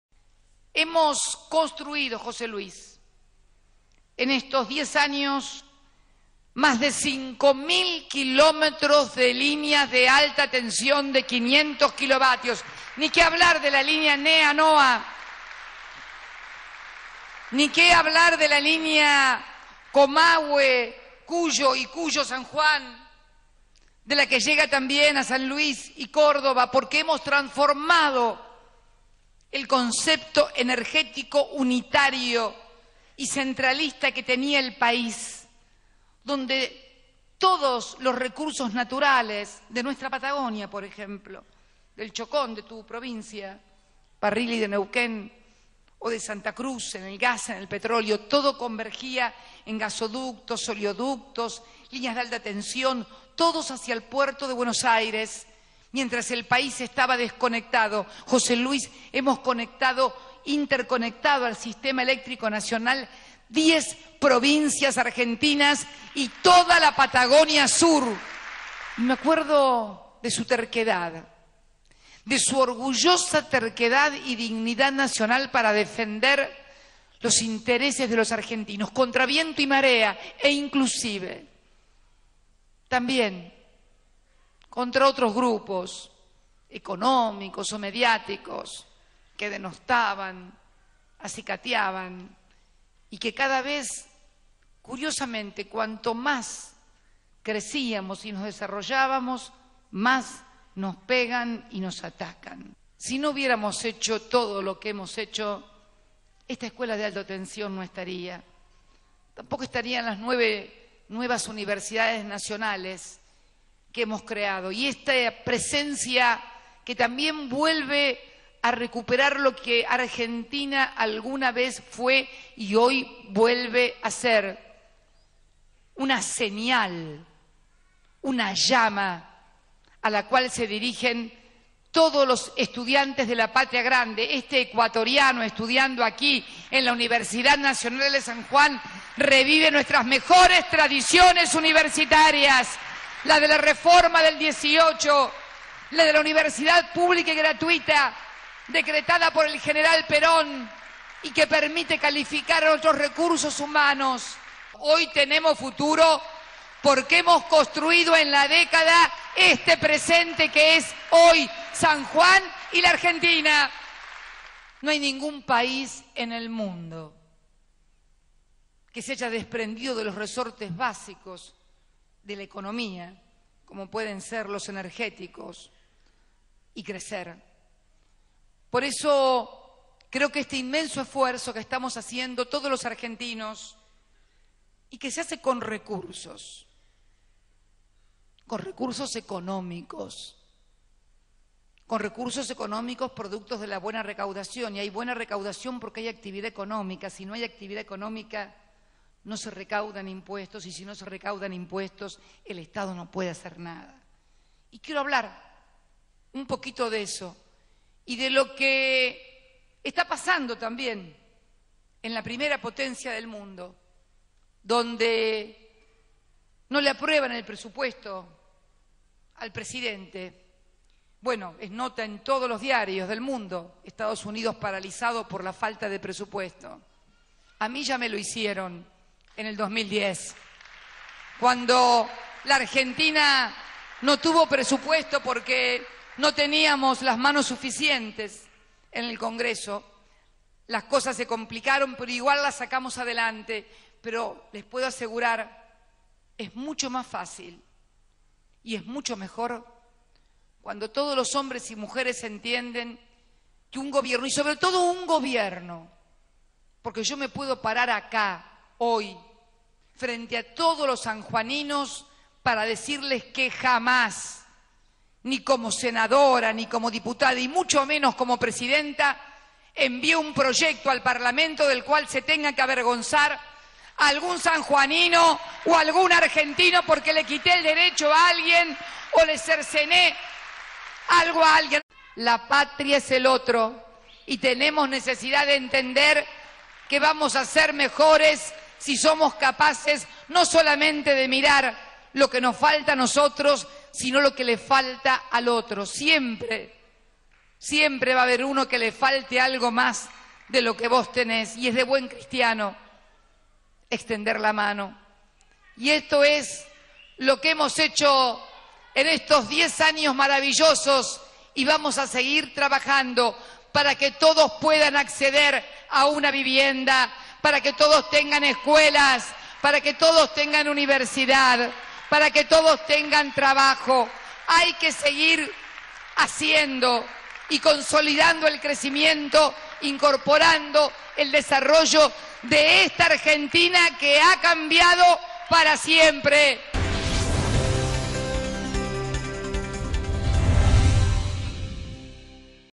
Cristina, en San Juan: "Hoy tenemos futuro porque hemos construido este presente"
La Presidenta aseguró que ésta "fue la década del crecimiento, pero ahora tiene que venir la década del desarrollo, que necesitamos apuntalar con inversión y presencia del Estado. Fue al inaugurar obras y entregar viviendas en San Juan.

Indicó que cuando más se desarrolla la Argentina "más nos pegan y nos atacan", y en ese sentido resaltó la figura del ex presidente Néstor Kirchner, al subrayar "su orgullosa terquedad para defender los intereses de los argentinos contra intereses económicos o mediáticos".

"Necesitamos la unidad de todos los argentinos para que lo que logramos no se desperdicie", remarcó la Presidenta.

También se refirió a la situación del gobierno norteamericano de Barack Obama, que atraviesa una difícil situación ya que la oposición republicana no le aprueba el Presupuesto.

"A mí me lo hicieron en 2010. No tuvimos presupuesto porque no teníamos las manos suficientes en el Congreso. Pero salimos adelante", remarcó.

En ese sentido, citó una frase de Obama, quien dijo "quieren llevar al default a los Estados Unidos" y se preguntó si lo mismo pretenden con la Argentina los fondos buitre.

Cristina afirmó que "la Argentina volvió a ser una señal, una llama a la cual se dirigen todos los estudiantes de la Patria Grande".

"Reviven nuestras mejores tradiciones universitarias", dijo la jefa de Estado.

En ese marco, Cristina resaltó la importancia de la Reforma Universitaria de 1918 y la gratuidad de la universidad pública dictaminada durante el gobierno de Juan Domingo Perón.

Cristina afirmó que esta "fue la década del crecimiento, pero ahora tiene que venir la década del desarrollo, que es la de la consolidación y de la profundización, con innovación tecnológica para todo el país".

En el marco de la entrega de 520 viviendas en San Juan, la mandataria se autocalificó como "una enamorada del progreso y crecimiento" y aseguró que "tiene que venir la década del desarrollo, articulando el país, donde podamos visualizar las ventajas competitivas de cada región y allí radicar la inversión".

Además, sostuvo que en la Argentina "hay buena recaudación porque hay actividad económica".

Cristina Fernández agregó que "hemos construido en estos diez años más de 5.000 kilometros de alta tensión" y "hemos transformado el concepto energético centralista que tenía el país".

"Hemos interconectado diez provincias argentinas y toda la Patagonia sur", afirmó.

La presidenta estuvo acompañada por el gobernador de San Juan, José Luis Gioja, además de funcionarios y legisladores.

Cristina también expresó que "hoy más que nunca necesitamos de la unidad de todos los argentinos".

"Hay un mundo complejo y difícil. Hoy más que nunca necesitamos de la unidad de todos los argentinos para el desarrollo nacional; para que todo esto que logramos no se desperdicie", dijo la mandataria.

La presidenta añadió que la Argentina será "mejor si somos capaces de mirar lo que le falta al otro" y llamó a consolidar el "crecimiento" del país "incorporando el desarrollo a esta Agentina que ha cambiado para siempre".